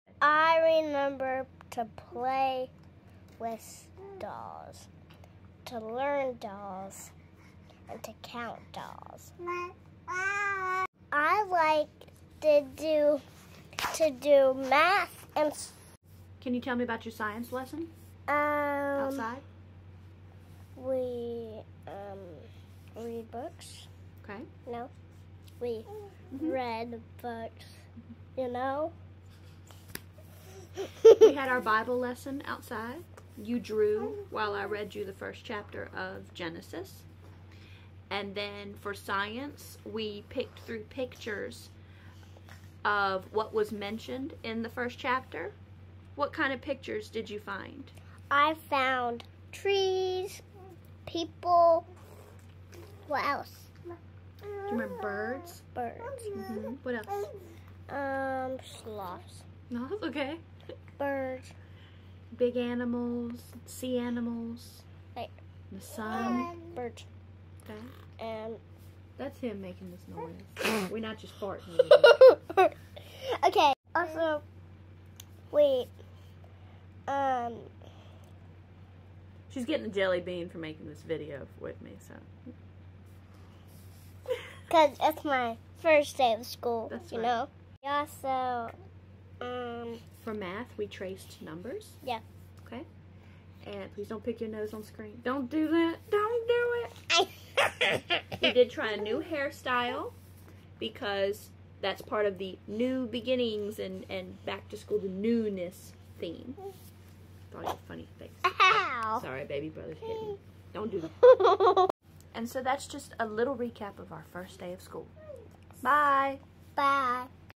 Our First Day of Homeschool
Amerisa gives a quick recap of her first day of Homeschool!